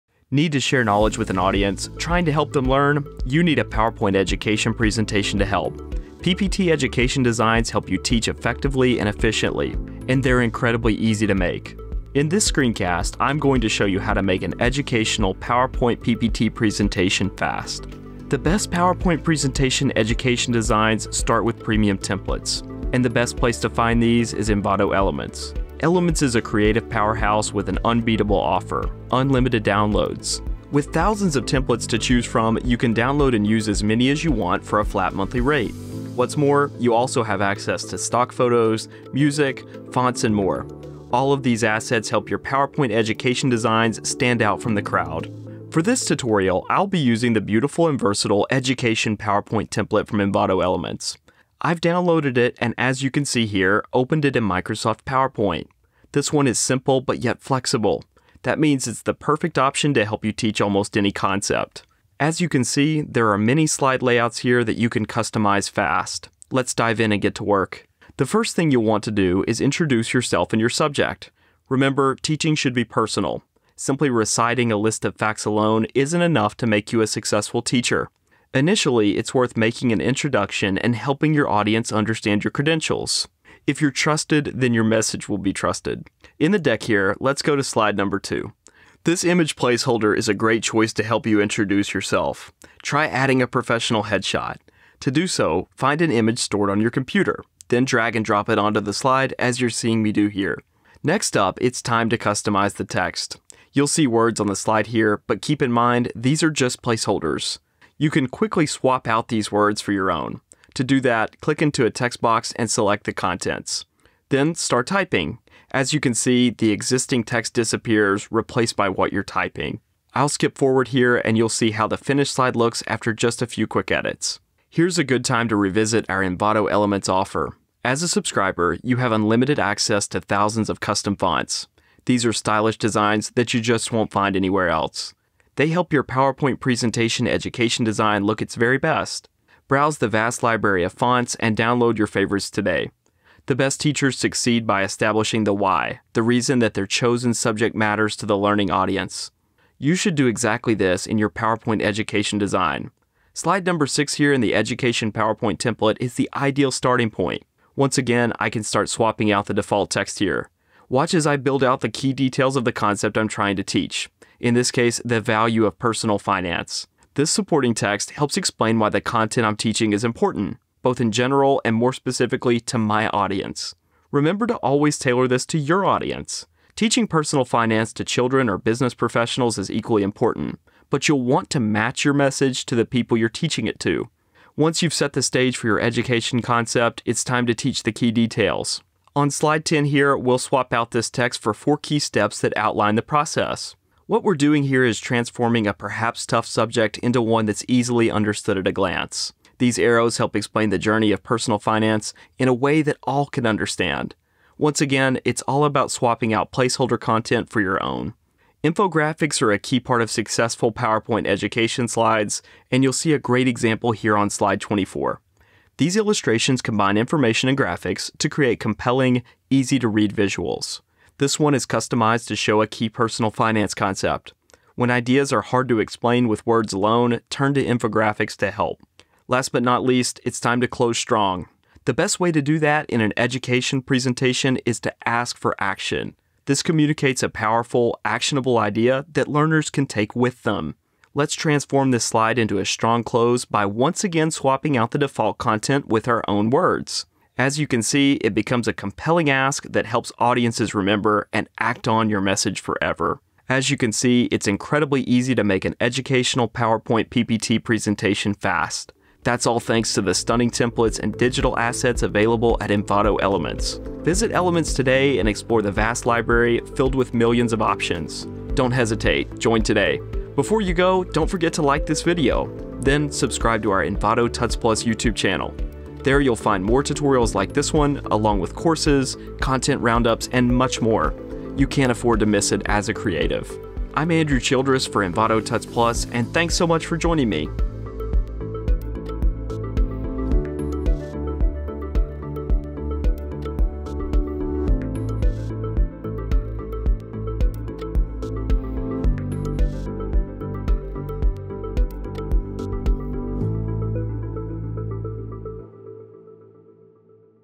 How to Make an Educational PowerPoint
Trying to teach a new concept to an audience? Hoping to show off your knowledge? A PowerPoint presentation education design is your best bet. These help you educate in style. In this video, you’ll learn how to make an educational PowerPoint PPT presentation fast.

Begin your PowerPoint education deck with an introduction. This is where your audience meets you and learns about your credentials. Add a photo of yourself by dragging and dropping it onto your slide. Then, simply swap out the placeholder text with your own words. This helps you start strong.

Any PowerPoint presentation education theme needs to answer the question “why?” You need to establish why the concept you’re teaching is important, and how your audience can relate to it. Dedicate a slide to this, explaining why your ideas matter.

Don’t forget to use charts and infographics to bring ideas to life. You can use these to explain numerical concepts, outline processes, and more. For example, you can map out steps in sequence with an arrow infographic.

Finally, you need to close strong. This is where you recap your PowerPoint education slides. Focus on what you've talked about, and end with a strong ask. This is a request or exhortation that learners will carry with them and act upon. Use it wisely!

Here's what the video covers:

0:00 Introduction
1:08 Create an introduction for your presentation
2:24 Create a class overview slide
3:10 Teach the class details
3:30 Use infographics
3:49 Close with a call to action
4:31 Conclusion

Read more on How to Make an Educational PowerPoint on Envato Tuts+

Envato Tuts+
Discover free how-to tutorials and online courses. Design a logo, create a website, build an app, or learn a new skill